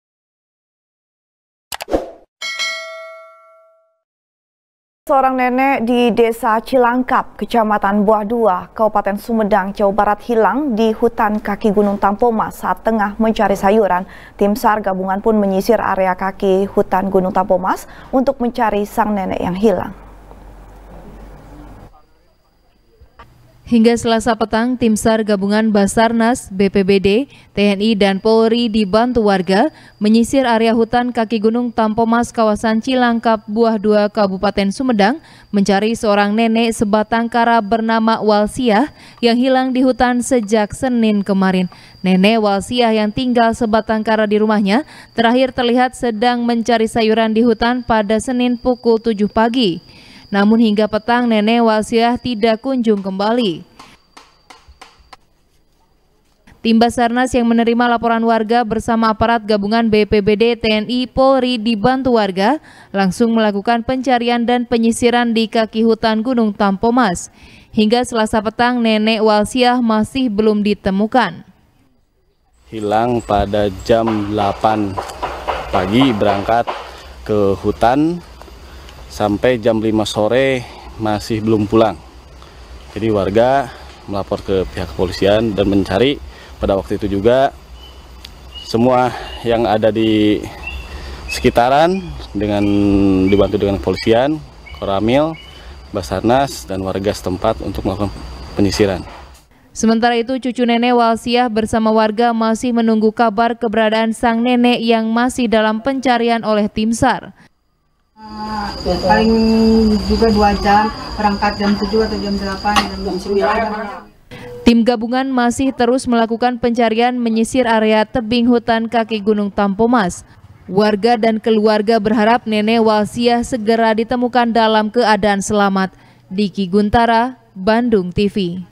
Cari Sayuran Di Hutan, Seorang Nenek Hilang Di Kaki Gunung Tampomas | SBR | BANDUNGTV
SBR | BANDUNGTV | FARHAT MUMTAZ | Seorang Nenek, Di Desa Cilangkap, Kecamatan Buah Dua, Kabupaten Sumedang, Jawa Barat, Hilang Di Hutan Kaki Gunung Tampomas Saat Tengah Mencari Sayuran. Tim Sar Gabungan Pun Menyisir Area Kaki Hutan Gunung Tampomas, Untuk Mencari Sang Nenek Yang Hilang.